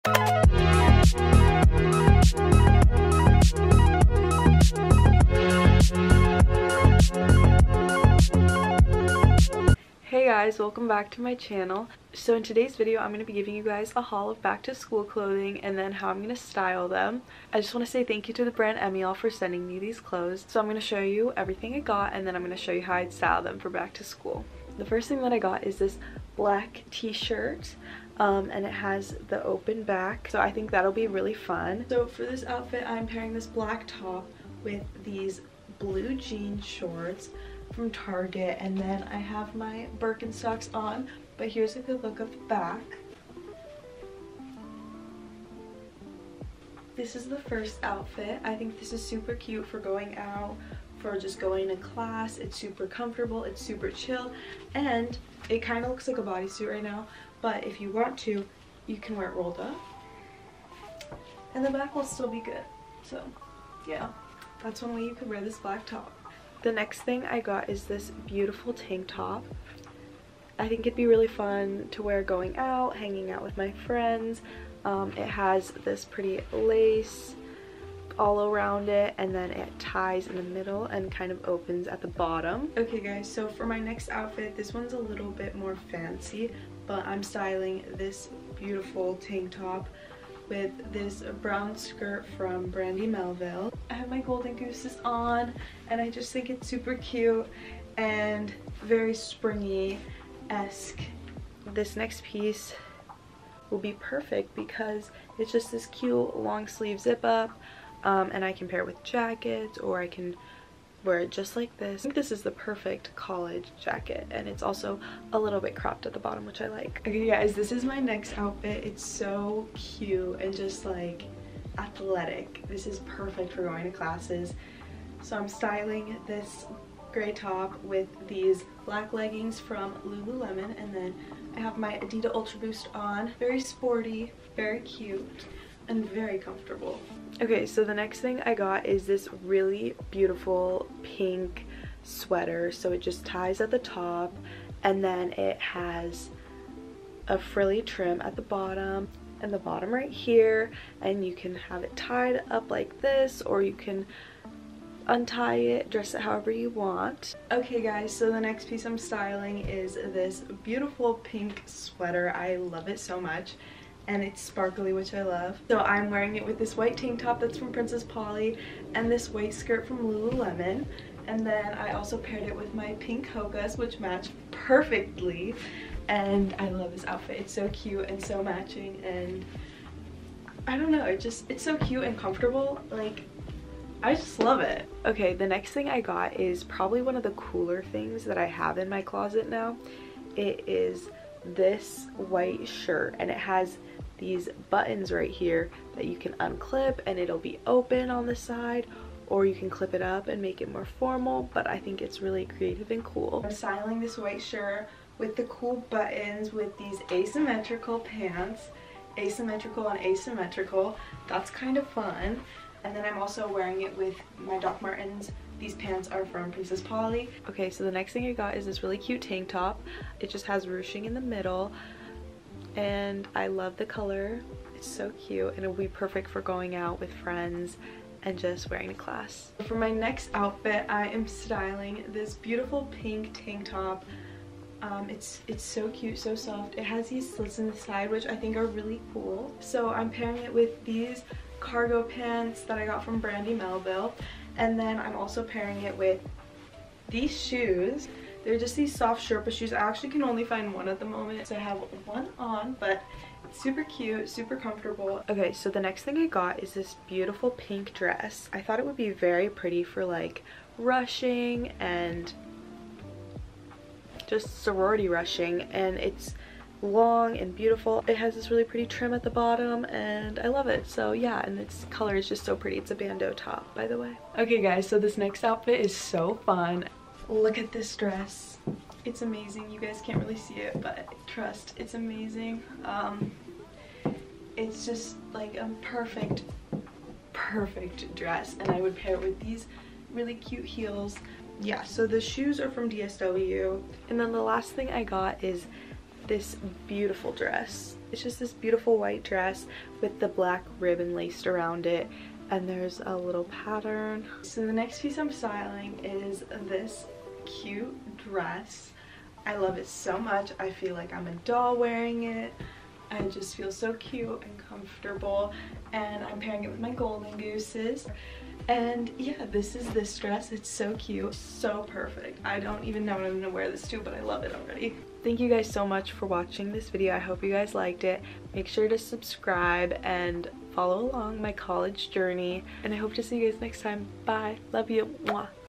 back to school clothing haul and outfit ideas!!
hope you guys enjoyed this video and got some outfit ideas for school coming up! Thank you to the brand emmiol for gifting me these clothes! you can use my code "lilylogan20" if you are looking for back to school clothes.

Emmiol Back-to-school sale
Sales Time：  July 15th to Aug. 31st

vsco: lilyylogann